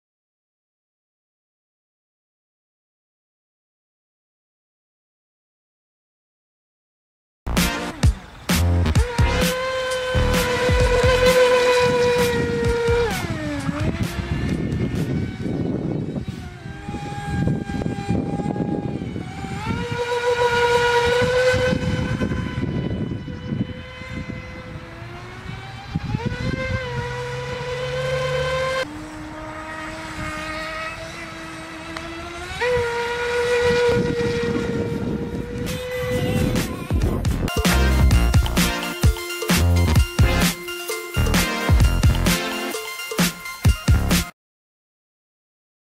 FT012 Speed Boat 🔥 RC Racing 🔥
🔥 RC Racing 🔥 RCRacing🇱🇰
💥THE HOBBY STORE 💥

Specifications
Model: FT012
Features: with Water Cooling Systerm,Large Size
What's in the box: charger,Batteries,Holder,Power adaptor,Remote Controller/Transmitter,Blades,1× User manual
Item Type: Remote Control Boats
State of Assembly: Ready-to-go
Channel number: 2
Battery: 6×AA
Running Time(Minutes): 10-15
Battery for Body: 1800mAh
Remote Control Distance(Meters): 100-150
Remote control type: 2.4G
Charging Time(Minutes): 180
Power Adapter Option: EU
Speed (km/h): 50
Motor: Brushless
Boat Type: Speedboat
Status: RTF
Material: ABS,ABS
Net Dimensions(cm): 46*12.5*11.8
Net Weight(kg): 1.75
Shipping Weight(kg): 1.85
Package Dimensions(cm): 55.0*22.0*20.0

💥Features & details

Equipped with a high power brushless motor and water cooling system

Super powerful and flexibly fast. Top speed can reach 50 Mph

Size Body Length: 18 in. Width: 5 in. Body Height: 5 in.

Boat Uses an 11.1V 1800mAh 25C LiPo Battery Included

Transmitter Uses 4 AA Batteries Not Included, Boat Range over 300 Ft.